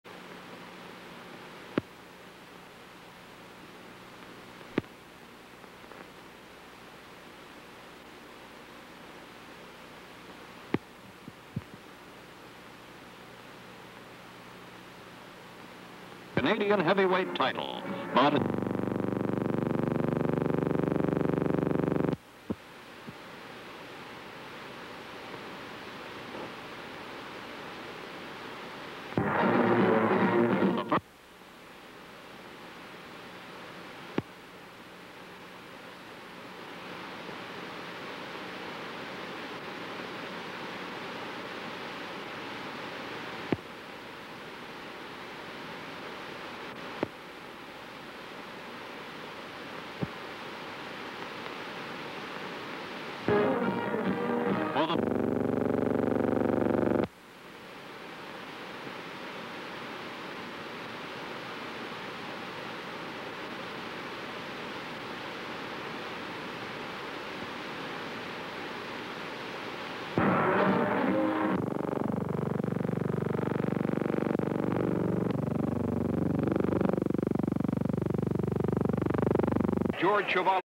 George Chuvalo vs Yvon Durelle
A rough fight between Canada's top sluggers George Chuvalo and Yvon Durelle.